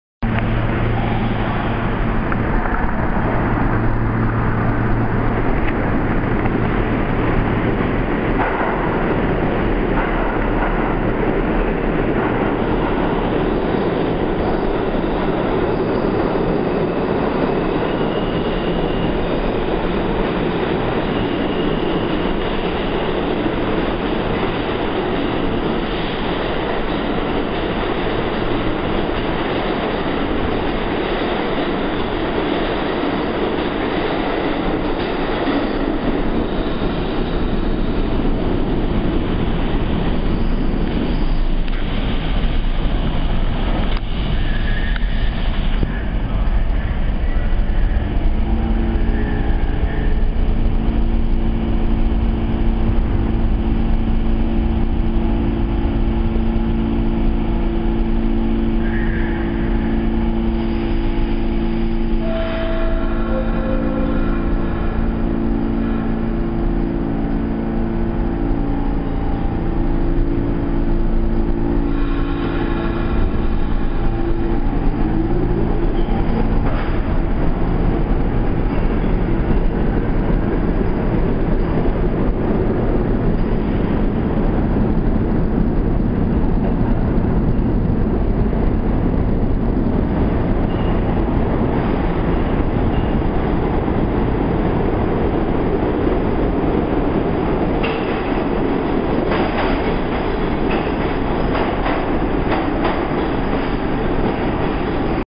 NYC Subway: 2nd Avenue-bound R46 (V) Entering & Leaving 36th Street
Taken on 6.21.10. (V) service ceased on June 26th, 2010 and replaced with the (M) from Broadway-Lafayette Street to Forest Hills-71st Avenue.